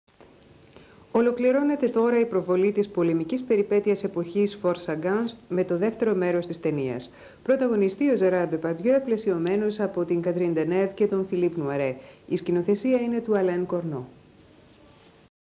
ΔΑΦΝΗ ΜΠΟΚΟΤΑ
Αναγγελία προγράμματος από την τηλεπαρουσιάστρια της ΕΤ-1
Δάφνη Μπόκοτα. Μεταδόθηκε Τρίτη 01 Οκτωβρίου 1991.